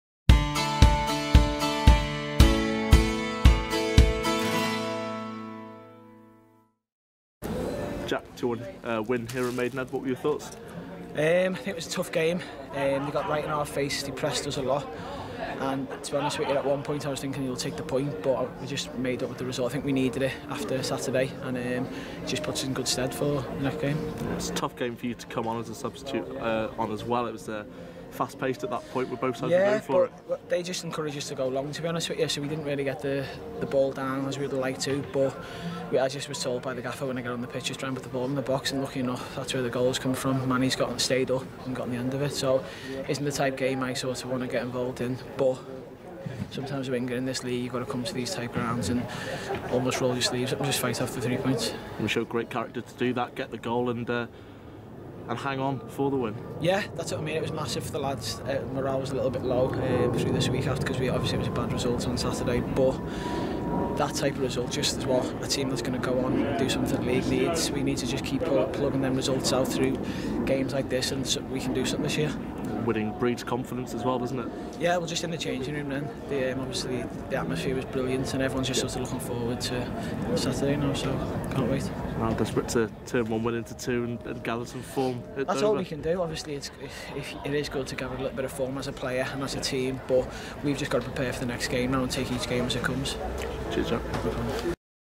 Jack Mackreth On His Shift At Maidenhead United
Another cameo performance from the bench by Jack Mackreth was fundamental for Wrexham's chances in the second half at Maidenhead.